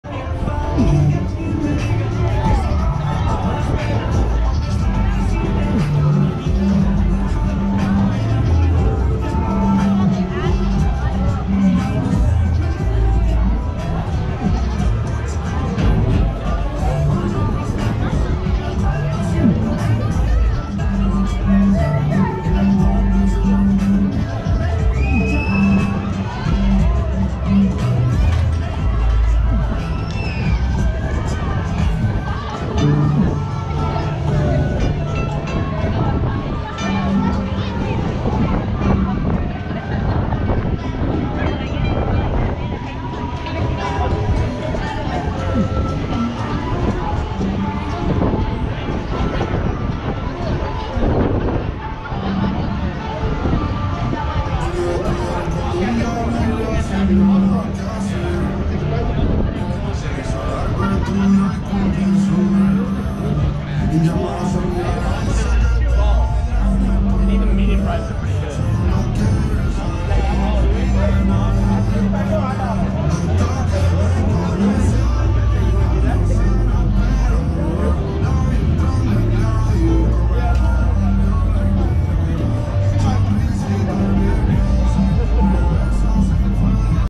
Sling Shot Ride (Off-Ride) August 6, 2023
Recorded on Sunday, August 6, 2023, 2:46:52 pm to 2:48:21 pm

A new ride at the OC Fair is called Sling Shot.